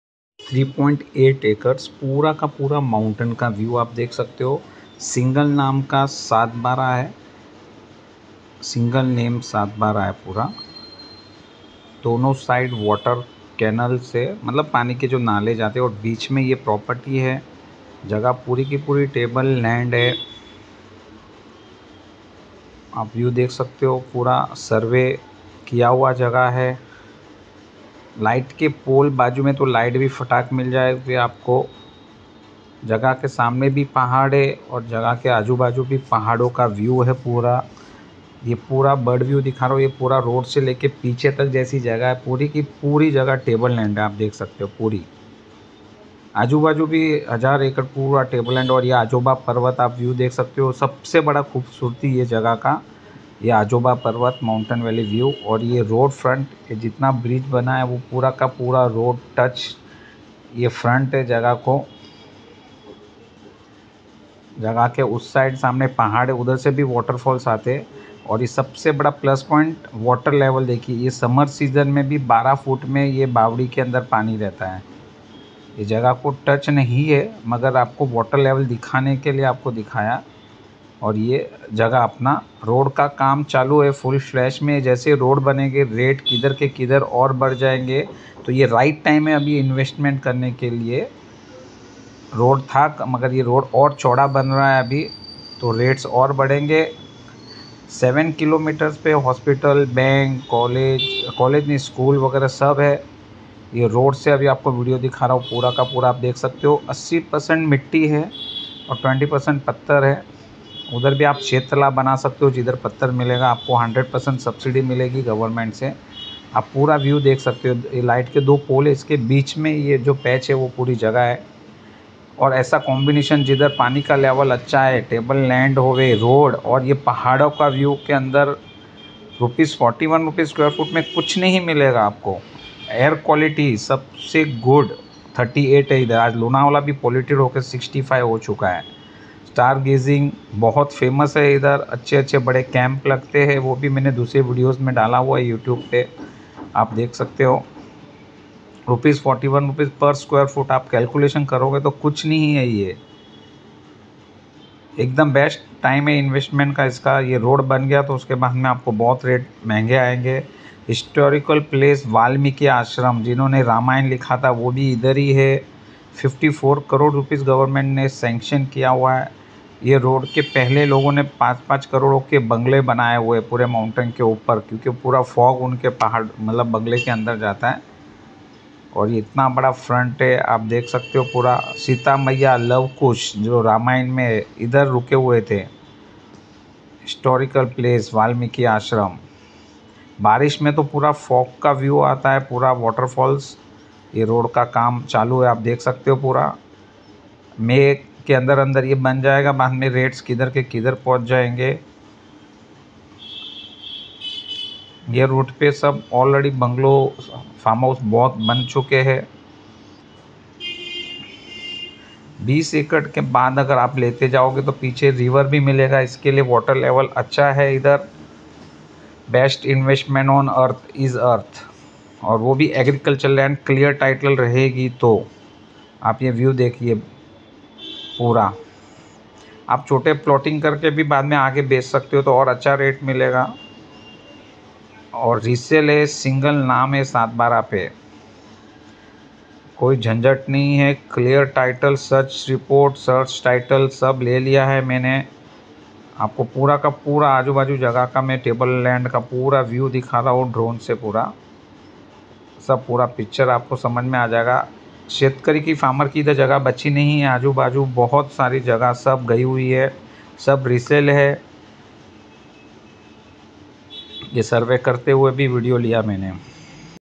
Rs 41 per Sqft,3.8 Acre SingleName 7/12, Mountain View,7 Km Hospital,Bank,Jain Temple,Big Road Front
*Rs 41 per Sqft 3.80 Acres with Secinic Mountain view totally Surrounded by Fog,Single name & Near River just at Rs 18 lkhs per acres.7 km Hospital,Bank, Market etc*

*Video link mentioned Below*

Totally Big Farmhouses near the Property

There is River also which u can in see  Drone Shoot,
Big Farmhouse near it who Has done Expenses more then 5 crores,
Totally Table land,
Single Owner Name on 7/12

*Beautiful Ajoba Mountain valley view & Becomes Fooggy in Rainy season*
Many more plots r there touching this property,
*Upcoming Resort after this property*
54 Crores Sanctioned by government to develop it,
 Totally Table land & Fertile soil...
Near Valmiki Ashram where Seeta ,Luv and Kush stayed in Ramayan..
Very Very Beautiful Place

90 km From Thane
110 km From Mumbai

*U can recover ur Money in 2 years by doing farming of Drumstick, Applebore n intercropping 🍉 etc etc*

*Agricultural land Benefits*
1)No Income tax,
2)Many Government Subsidy,
3)Always Appreciation,
4)Safe then Bank or Gold,
5)Can be used as Farmhouse in such Corona Virus,
6)Generate Income by doing Organic farming.

*Please Note*
Google location and Video link Mentioned in message